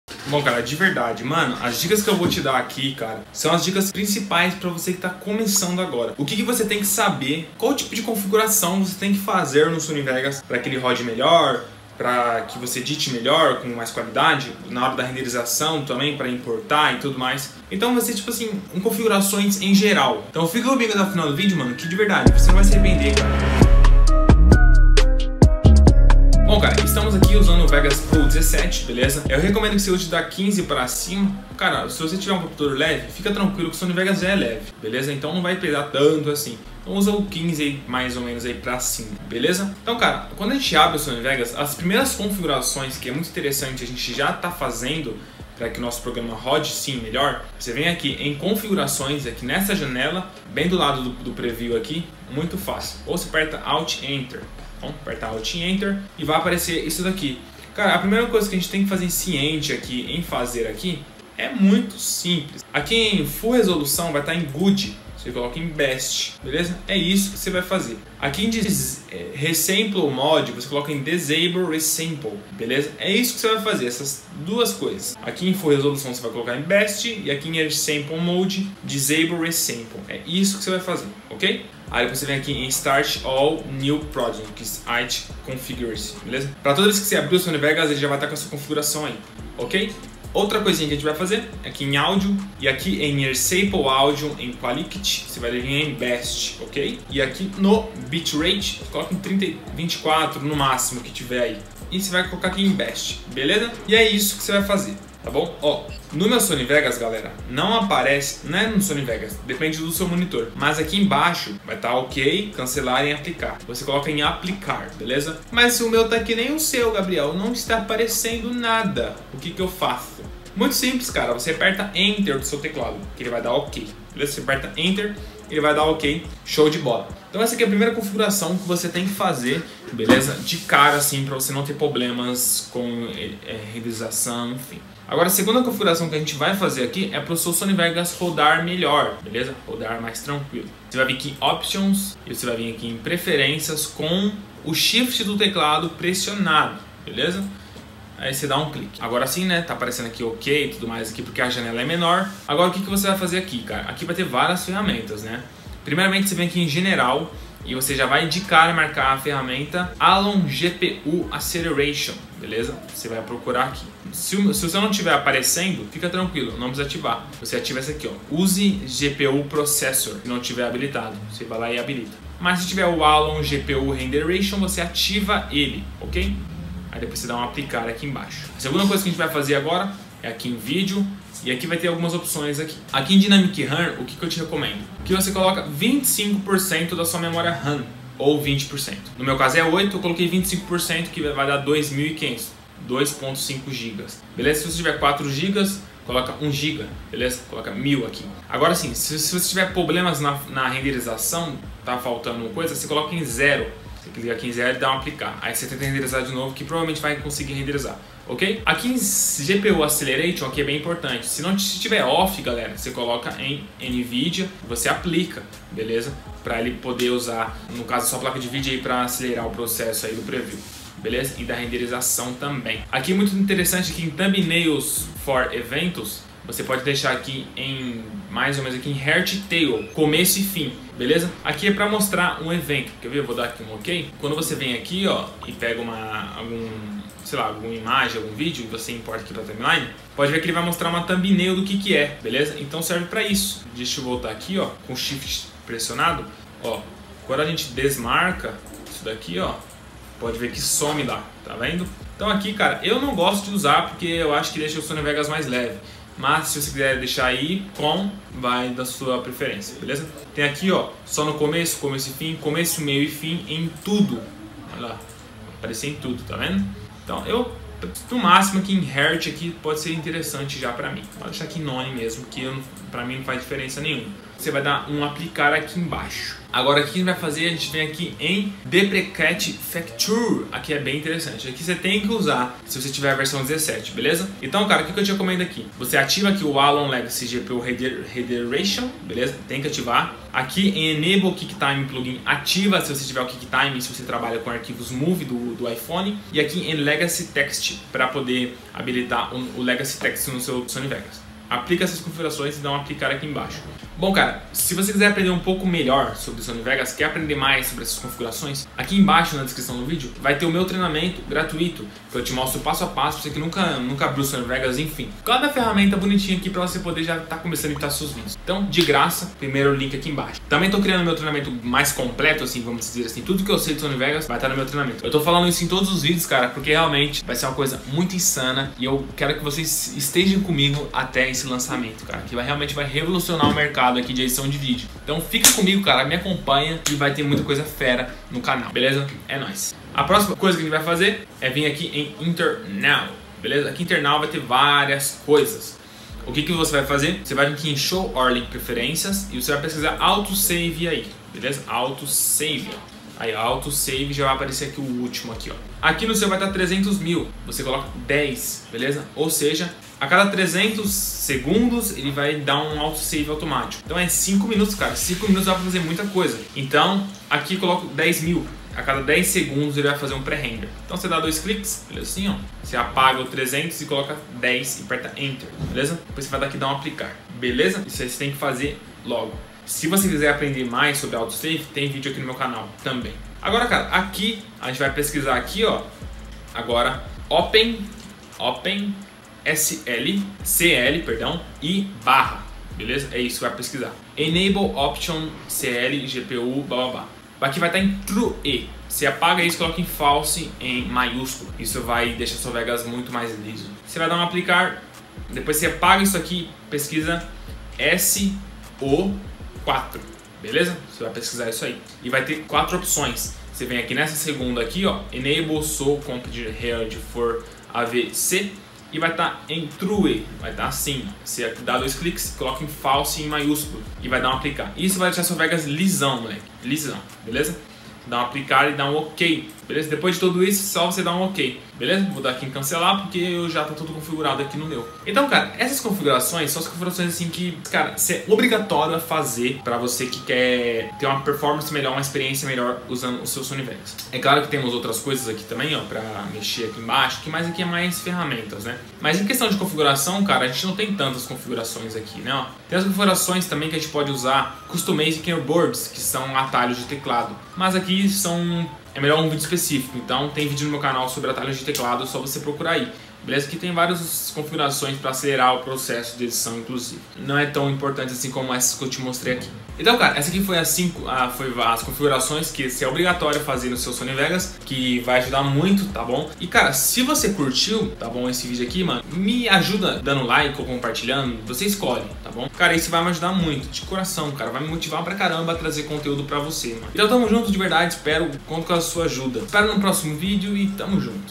Melhores CONFIGURAÇÕES OBRIGATÓRIAS que você tem que SABER no Sony Vegas (GUIA COMPLETO INICIANTE)
COMO CONFIGURAR O SONY VEGAS PARA MELHOR DESEMPENHO E QUALIDADE.

NESSE VÍDEO (PESQUISAS RELACIONADAS)

Nesse vídeo te mostro que o sony vegas tem várias configurações que todo usuário de vegas pro deveriam saber,é sério,essas configurações são ótimas para aumentar o desempenho e qualidade do sony vegas,e também vão te ajudar a editar mais rápido e fazer que sua edição seja mais completa com qualidade.

Então no tutorial de vegas pro de hoje mostro a você que se você tiver um computador mais inferior para rodar o sony vegas,não ficar de fora e configurar ele da melhor maneira possível pra ter um desempenho legal ai no seu vegas pro.

E é claro que também nesse vídeo não te dou apenas essas dicas de melhor o desempenho e qualidade,te dou também dicas para fazer determinados efeitos ou até animações,configurações também de projeto para você que é iniciante no vegas pro também.